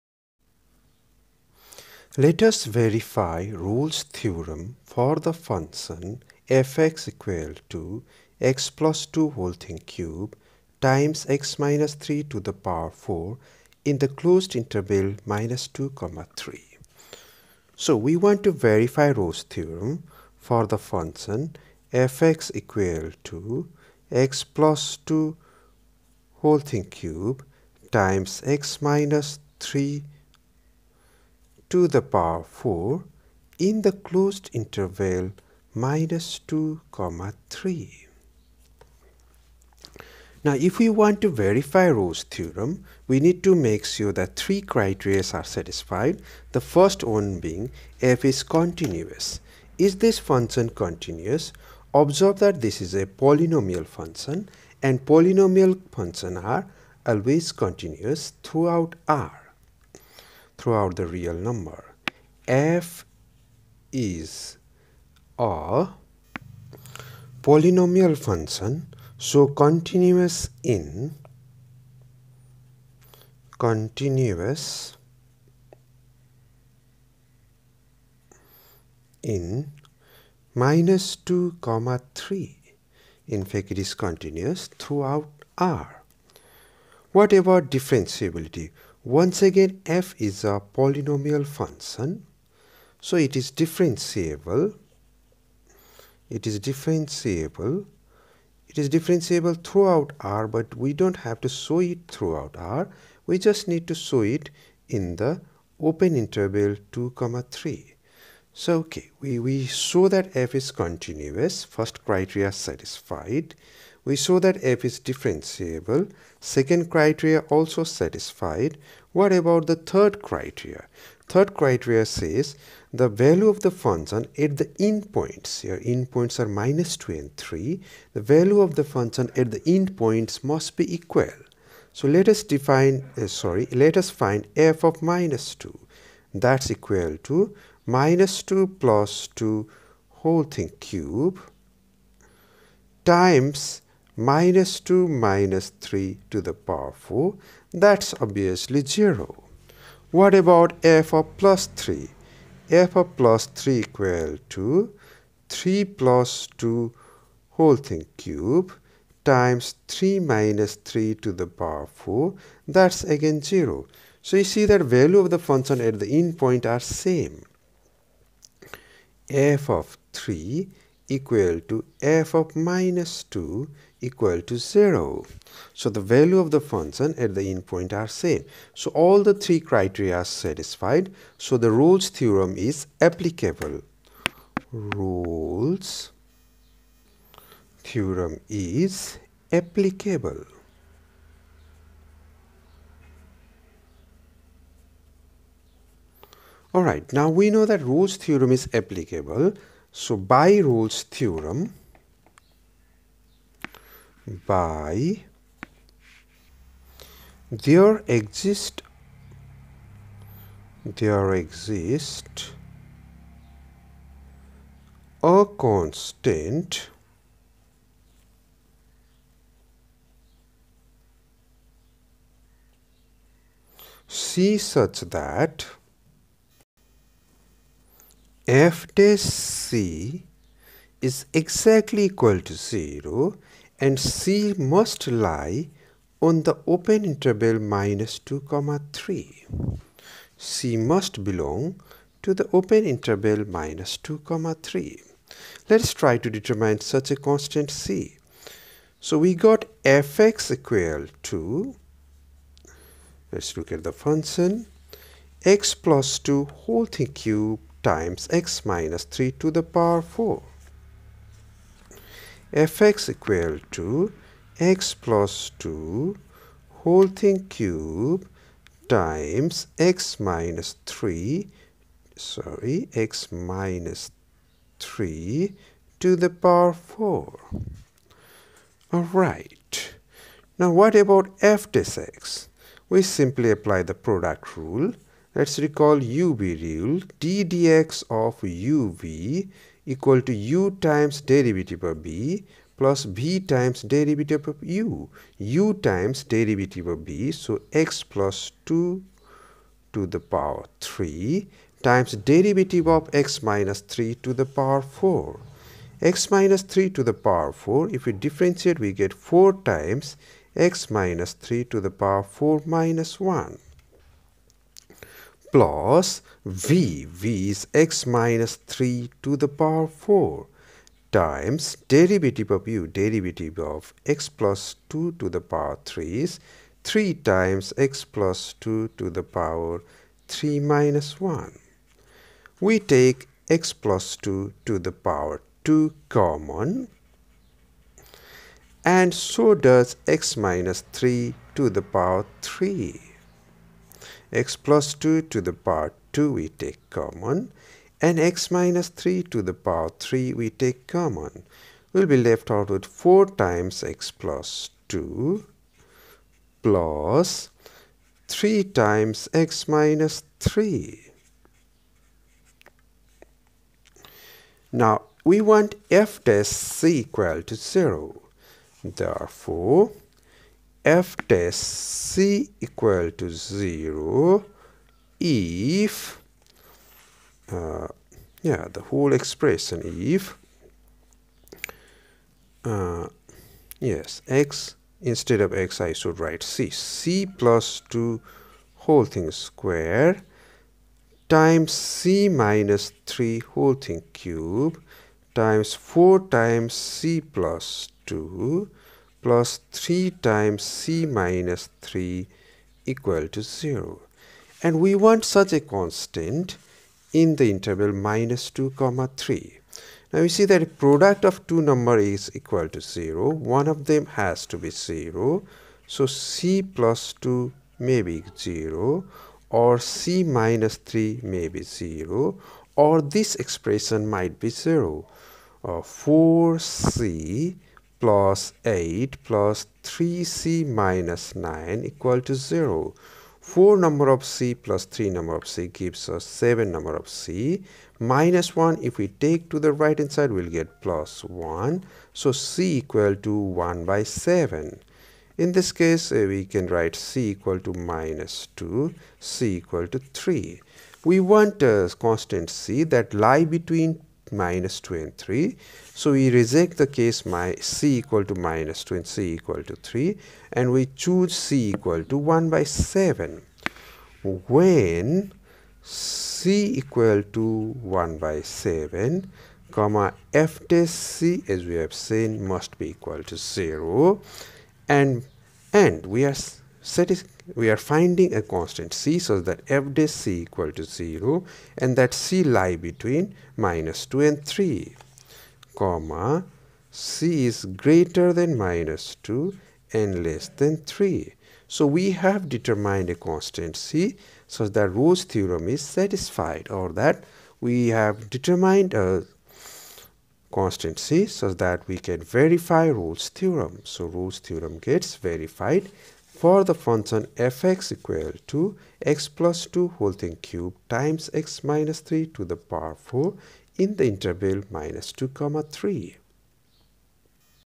Verify Rolle's theorem for f(x)=(x+2)^3(x-3)^4 in [-2,3]
We're  verifying Rolle's Theorem for the given function.